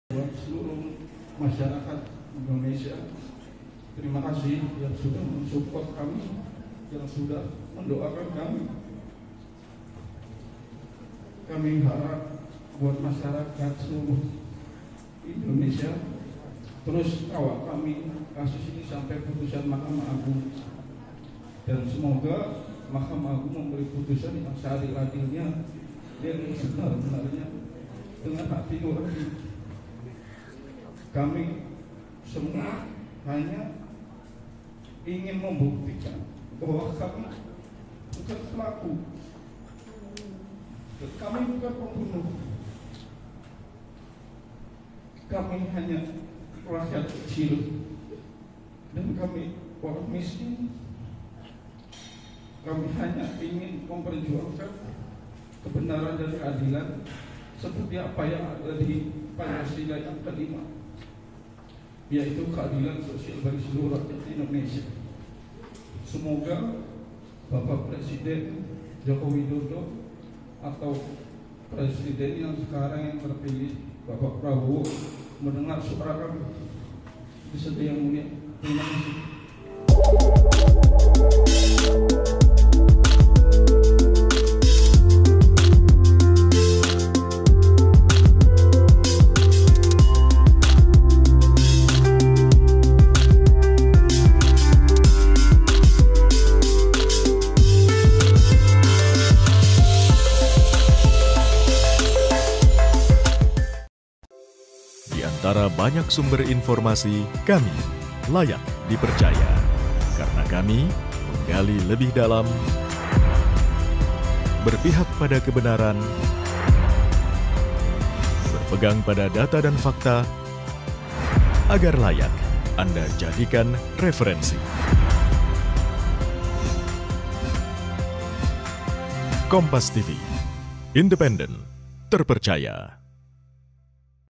Pesan Terpidana Hadi Tutup Sidang PK, Singgung Jokowi hingga Prabowo
CIREBON, KOMPAS.TV  Salah satu terpidana kasus Vina, Hadi Saputra menyampaikan pandangannya menutup sidang PK enam Terpidana pada Jumat (27/9/2024)

Hadi mengatakan bahwa dirinya hanya ingin membuktikan dirinya dan 6 terpidana lain bukanlah pelaku pembunuhan.

Hadi juga mengucapkan terima kasih kepada Peradi, Seluruh Masyarakat, Media massa yang telah membersamai untuk mencari keadilan.

Dirinya juga berharap kepada Presiden Jokowi hingga Presiden Terpilih Prabowo Subianto untuk mendengar prmohonan mereka.